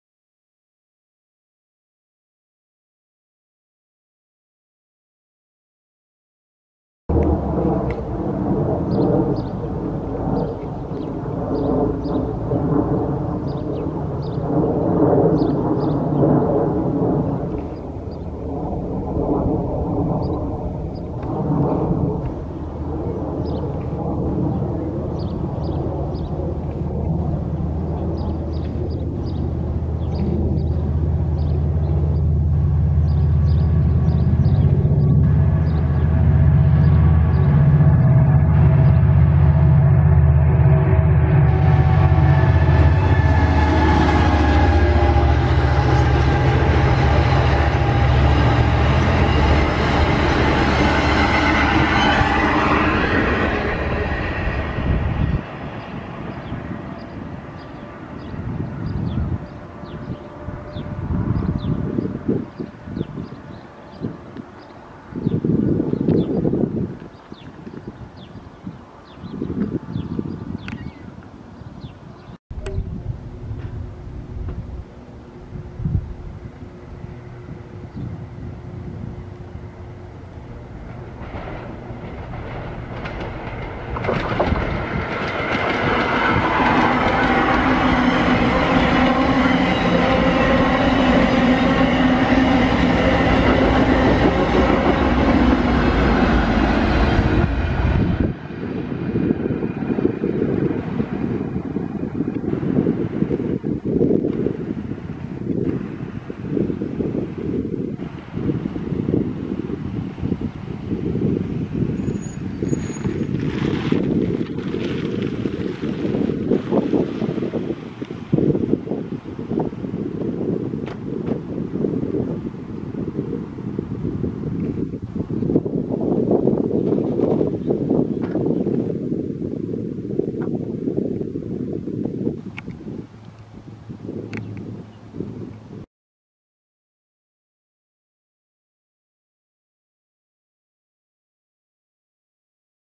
le mardi le 3 avril 2018 Sortie Railfanning dans la ville de Montréal
le lundi le 2 avril 2018 et j'ai passé une très belle journée là bas et j'ai filmer un train de l'AMT (RTM) et dans la rue Guizet, dans l'île de Montréal et dans la ville de Montréal, QC Canada (vidéo prise par ma caméra Canon)